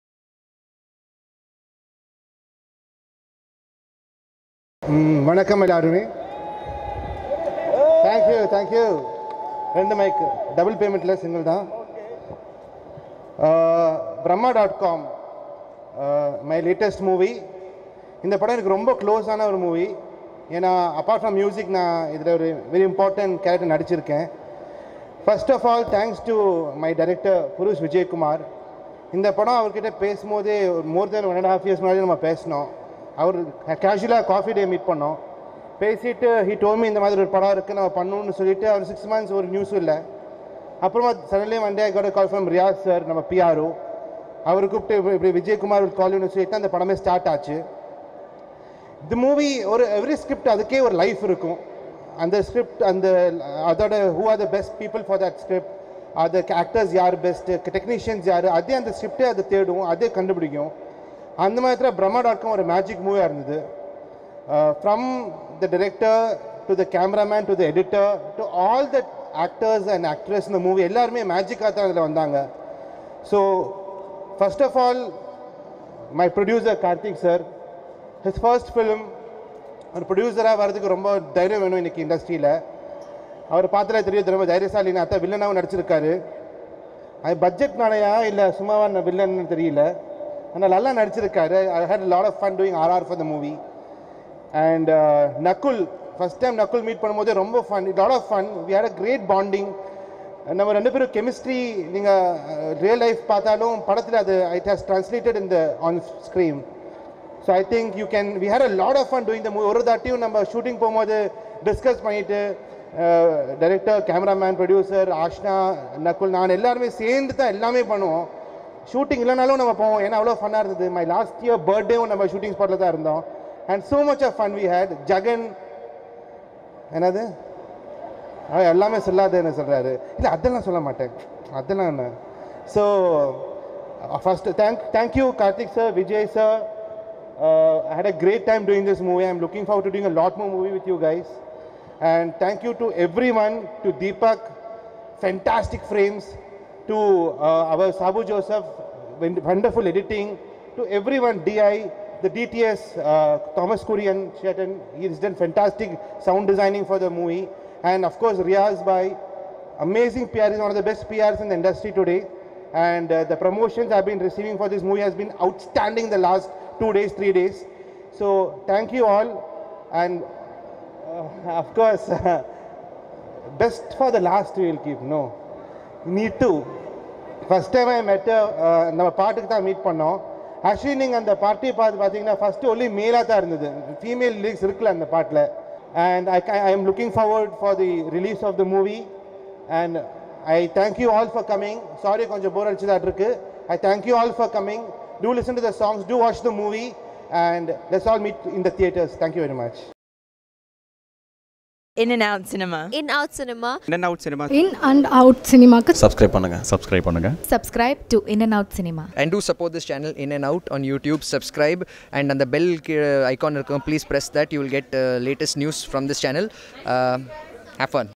"My Bond With Nakul Is Very Strong" Siddharth Vipin | Brahma.com Movie Audio launch
Brahma.com Audio Launch
Directed by - P. S. Vijayakumar
Starring  - Nakul
                  Ashna Zaveri
                  Siddharth Vipin